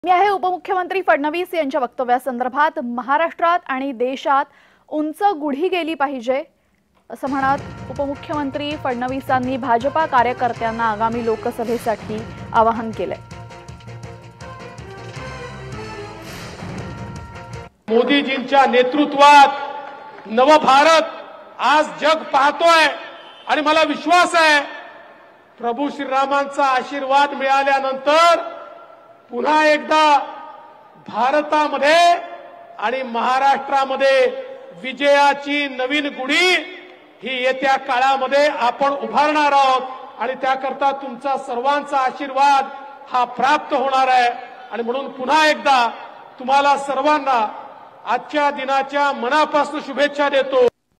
Devendra Fadnavis । 'महाराष्ट्रात आणि देशात गुढी उंच गेली पाहिजे' । Marathi News
'महाराष्ट्रात आणि देशात गुढी उंच गेली पाहिजे'
उपमुख्यमंत्री फडणवीसांचं भाजपा कार्यकर्त्यांना आवाहन